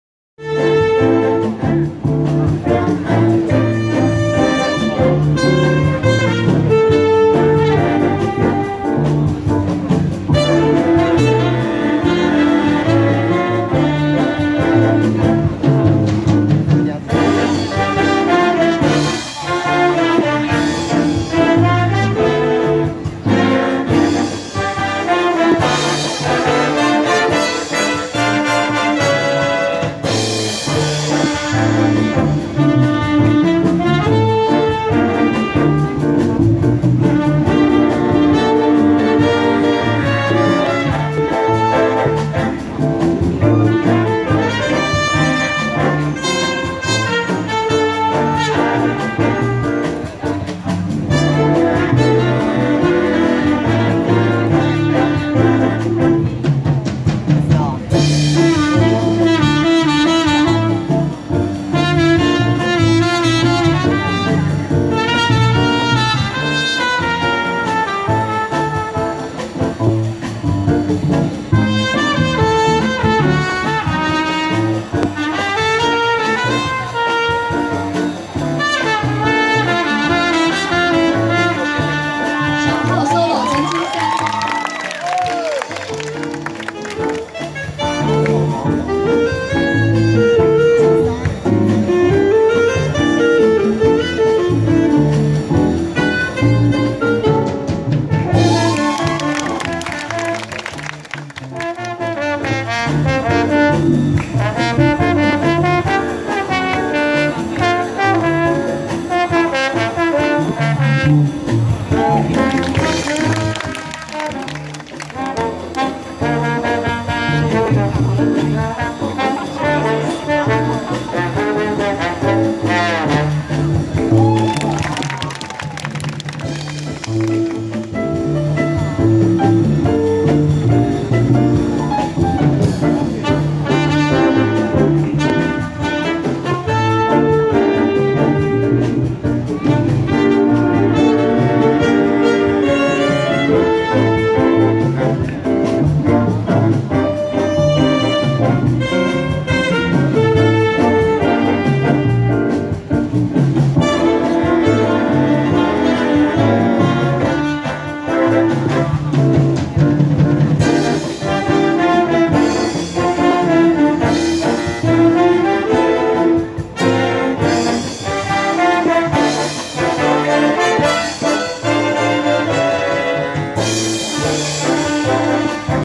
清水高中音樂班-爵士大樂團
2015台中爵士音樂節